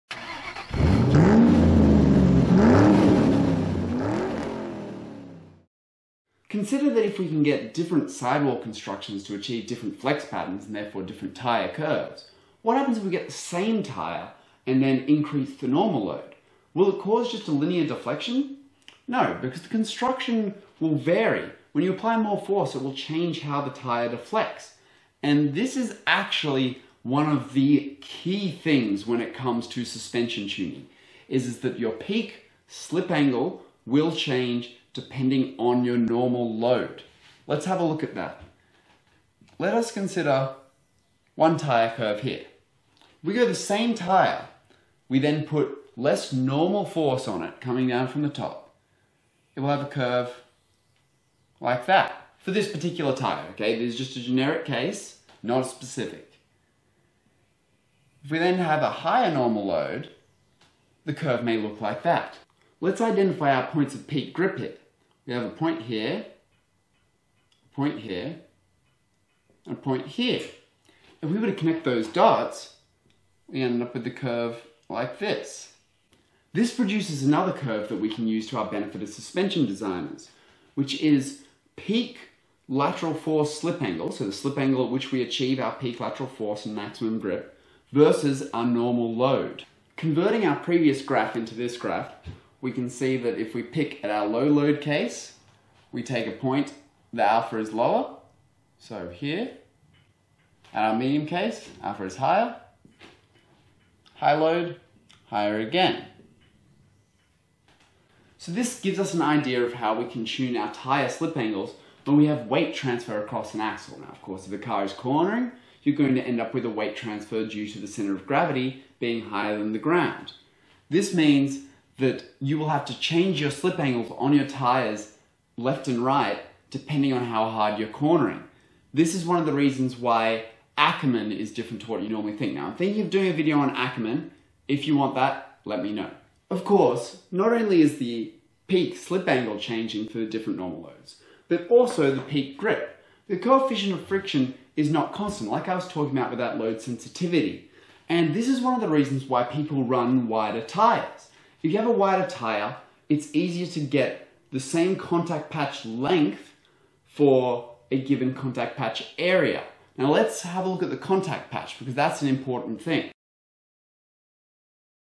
Slip Angle Load Sensitivity Explained
This is Part 5 of my 10 part series on how race car tires/tyres work.  This part covers what happens to slip angle on a tyre when the normal load increases.